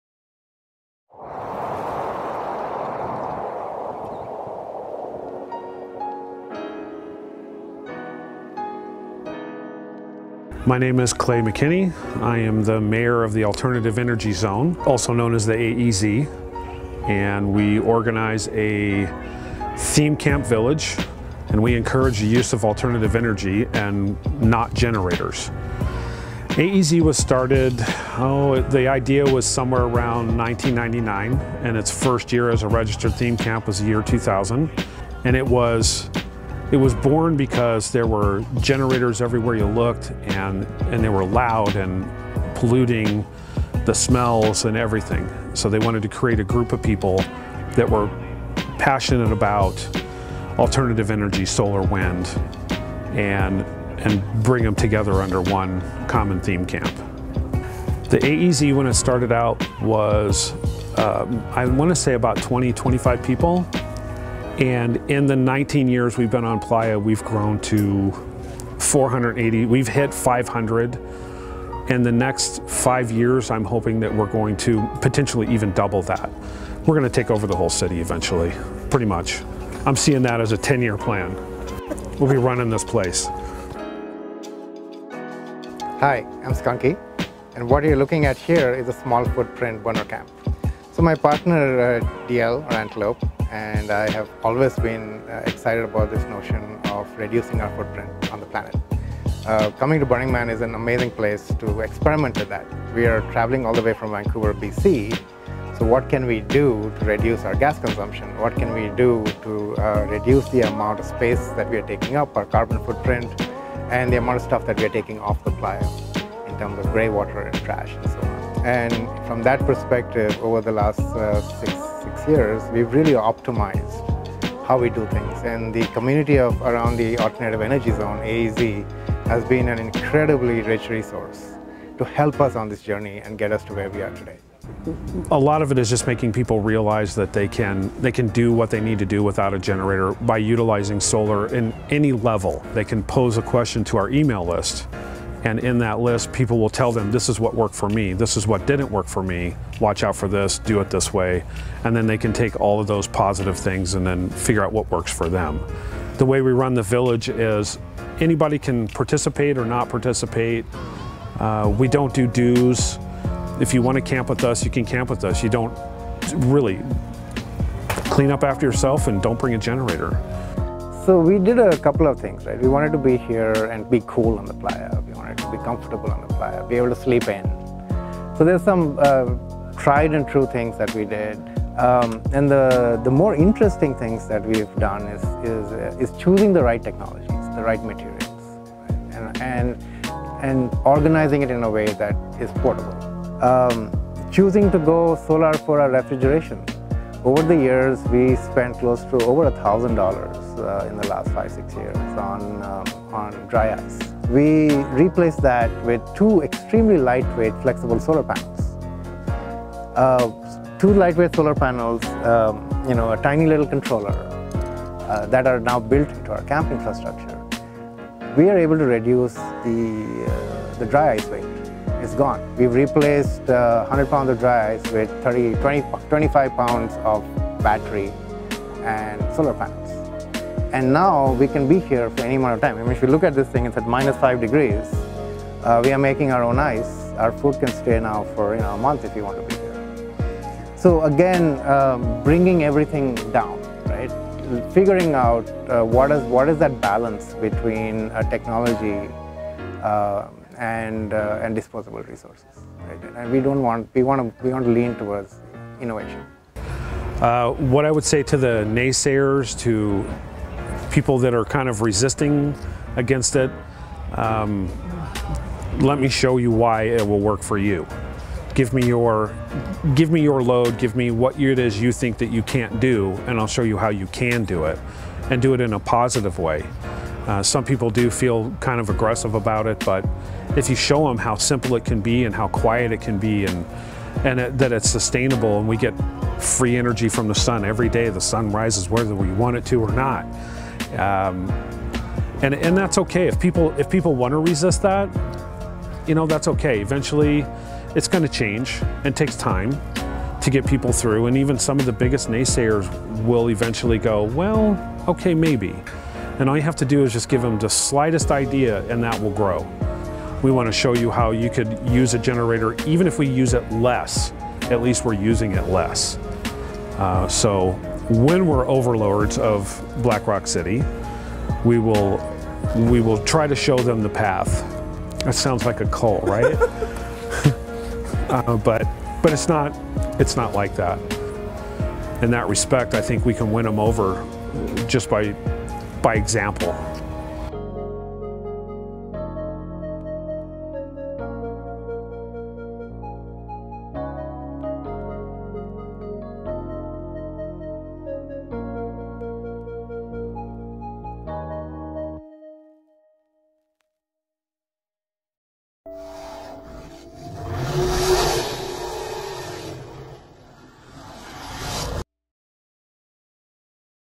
Burning Man Theme Camp Innovators: Alternative Energy Zone (AEZ)
The third installment in the Theme Camp Innovators series from Profiles in Dust covers the Alternate Energy Zone (AEZ), a Burning Man village devoted to generator-free living.